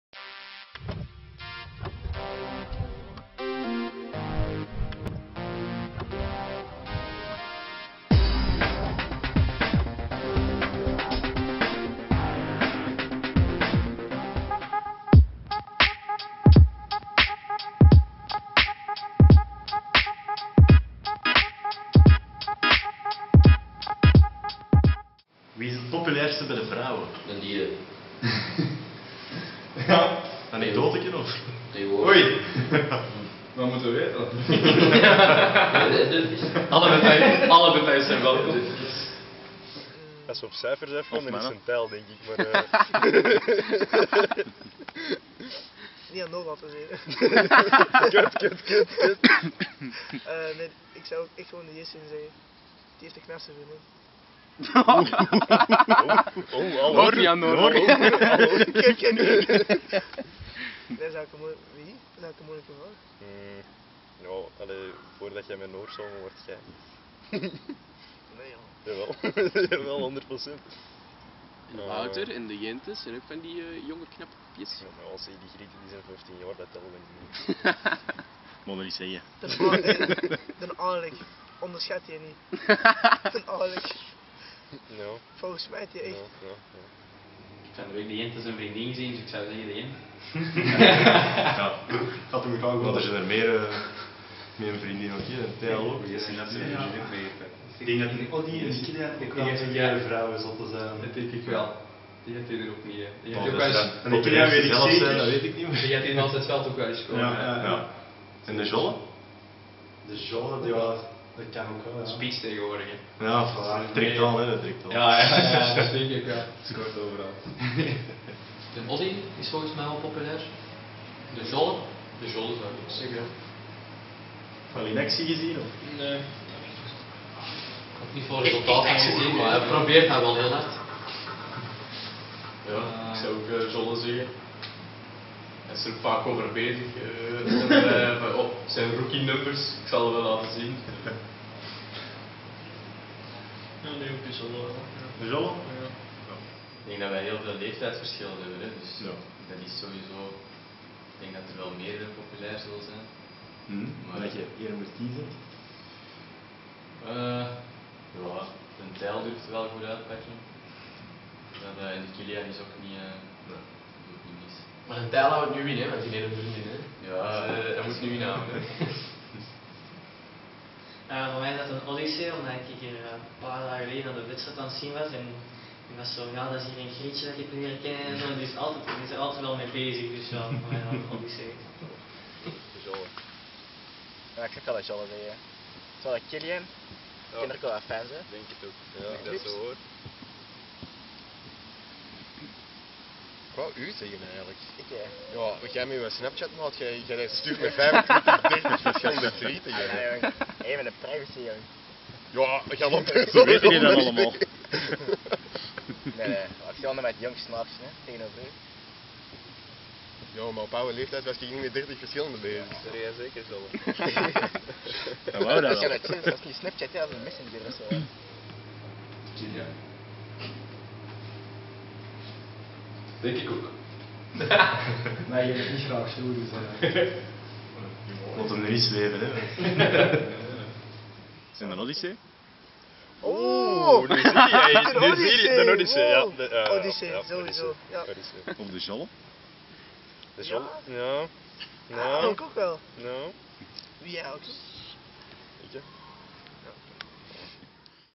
Mogen wij U voorstellen: De FUTURES #11
De Futures (4de Provinciaal) en de Eerste Ploeg (3de Provinciaal) van Excelsior Mariaburg hebben het idee opgevat om zich voor te stellen aan jullie.
Dit is #11 van de Futures.
Ook sportief een leuke bende en ze zullen een bezoek aan hun wedstrijden zeker op prijs stellen.
Met dank aan Yarno voor de montage.